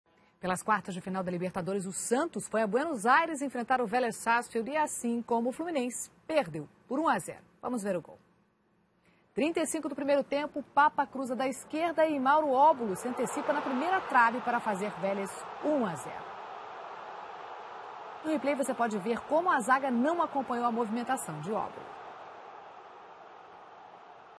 Santos perde para Vélez por 1 x 0 na Libertadores 2012 - 17/05/2012
Com a derrota, o Santos precisará ganhar por dois gols de diferença no jogo de volta para conseguir a classificação para a semi da Libertadores 2012.

Fonte SPORTV